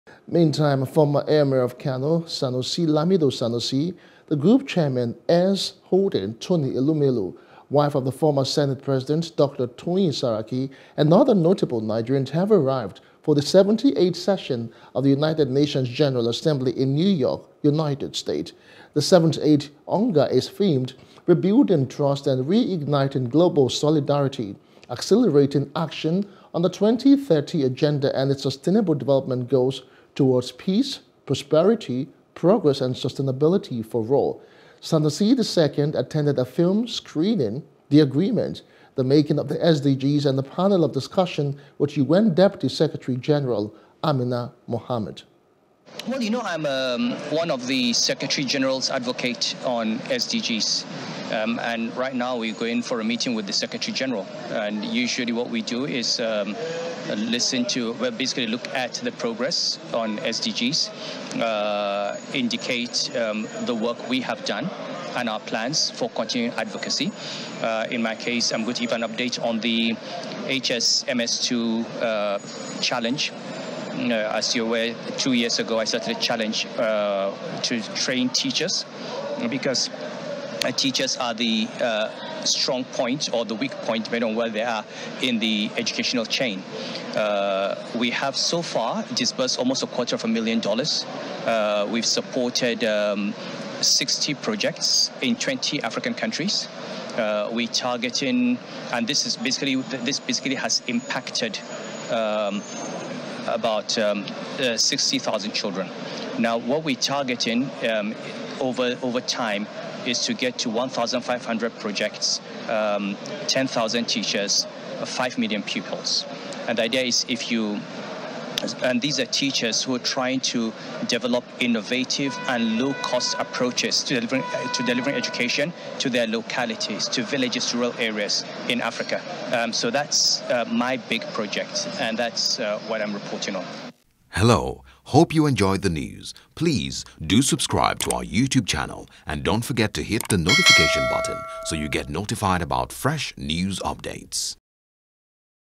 UNGA: Ex-Emir Sanusi Ii, Arrive New York To Speak On SDGs
A former Emir of Kano, Sanusi Lamido Sanusi; Group Chairman, Heirs Holdings, Tony Elumelu; wife of the former Senate President, Dr. Toyin Saraki, and other notable Nigerians have arrived for the 78th session of the United Nations General Assembly (UNGA) in New York, United States.